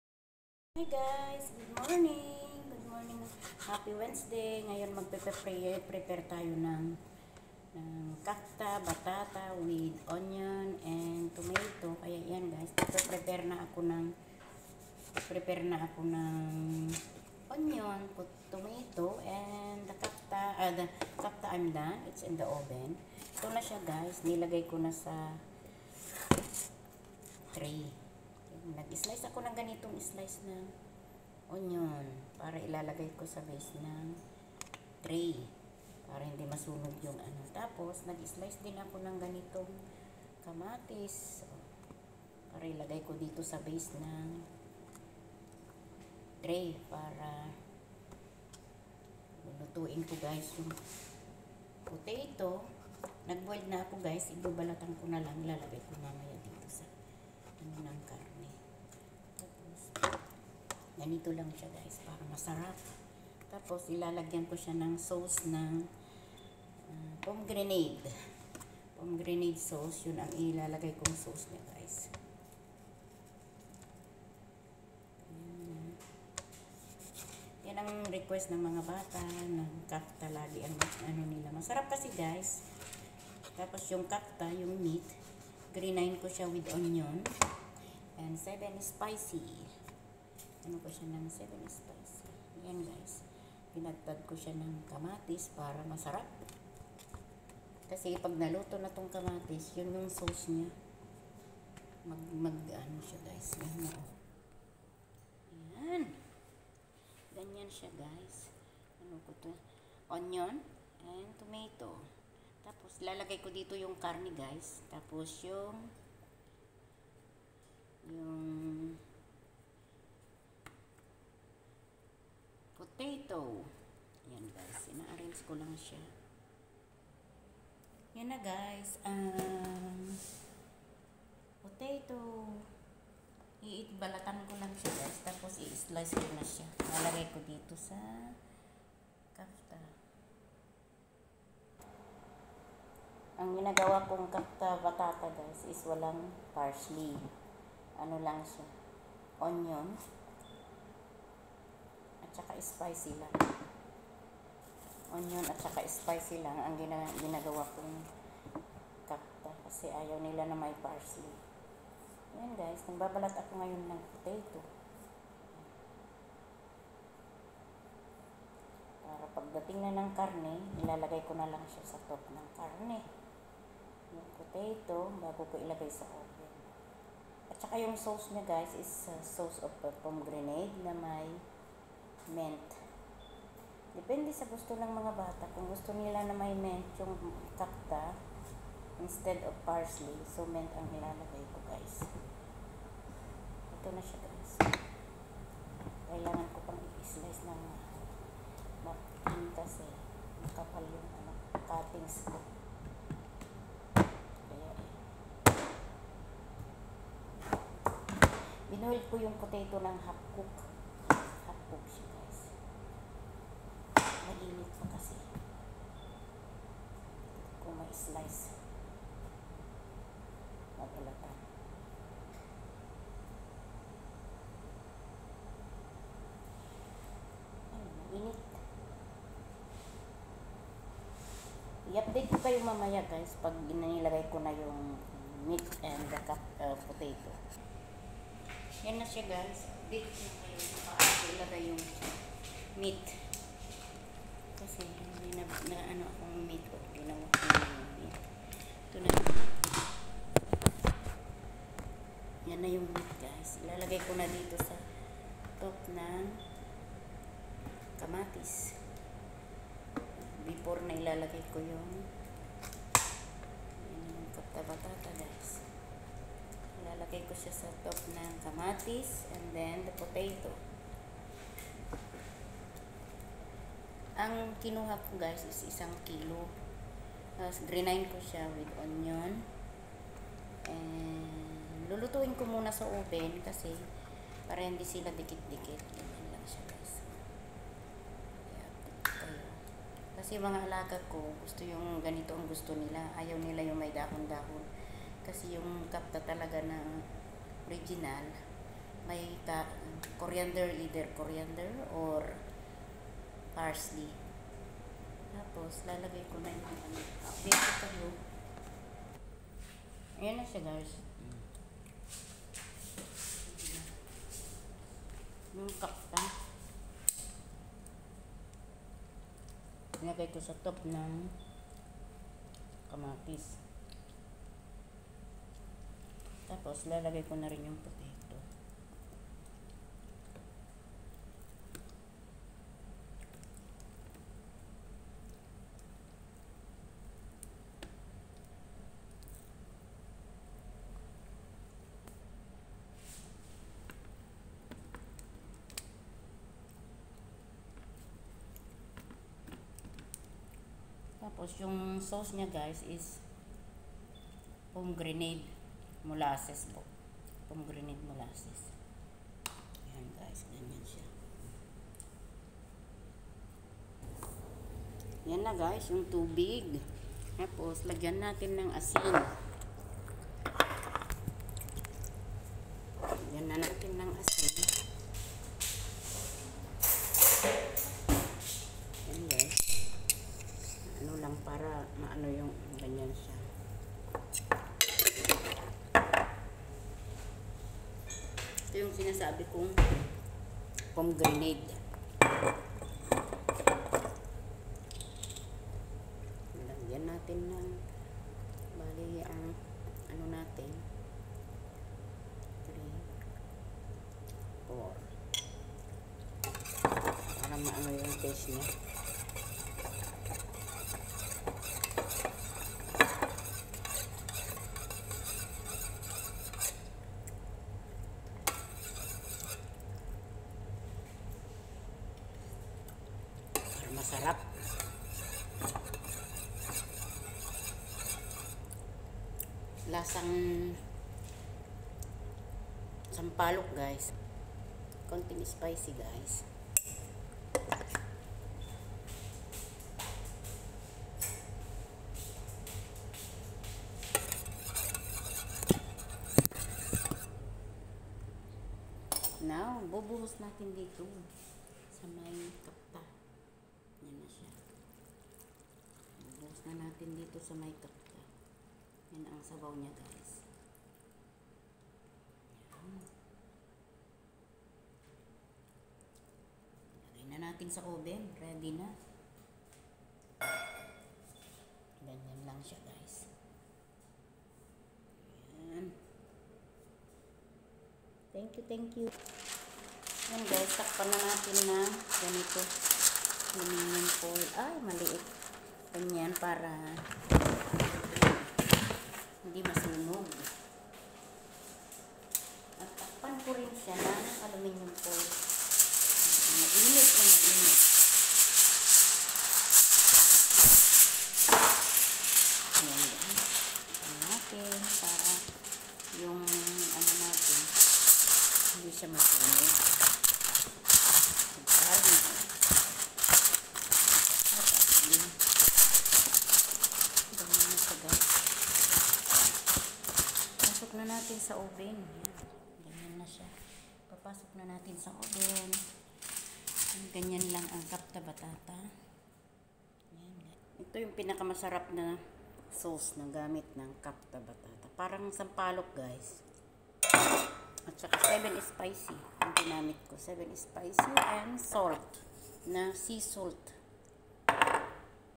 another ways for cooking kafta batata in the oven
another ways to cook kafta batata with pomegranate sauce